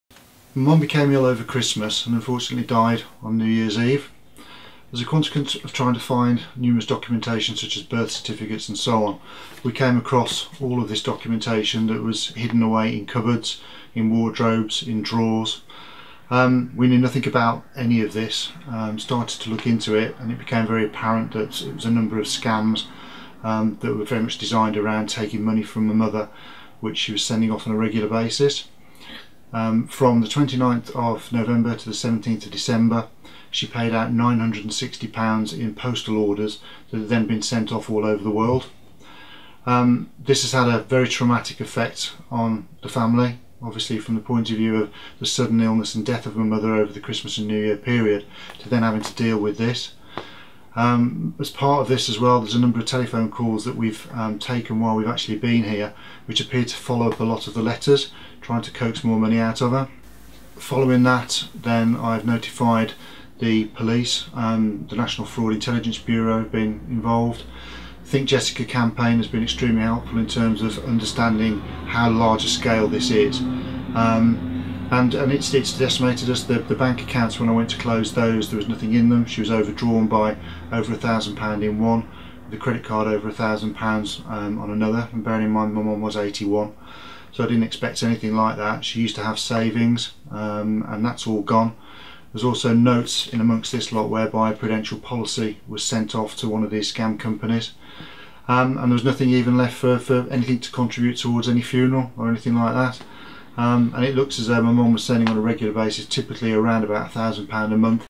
Grieving sons horror at discovering late mother conned out of £100,000 by postal scammers
Tragic Vicki Westwood, 81, was deluged with an average of 100 letters a week for SIX YEARS and backed up with dozens of phone calls from fraudsters claiming she had won a cash prize.  The vulnerable widow was persuaded to send money, supposedly for administrative fees, to dozens of fake companies with the promise she would receive her reward.
Shockingly, when she tried to stop sending the crooks the money, she was plagued with phone calls from the conmen who pressured her into posting them more cash.  Between 2008 and when she died on New Year’s Eve, she had been swindled out of more than £100,000 – an average of £1,000 every week.  The extent of the fraud was only uncovered when her son Russell Eaton, 50, was going through her bank accounts after she died and discovered she was broke.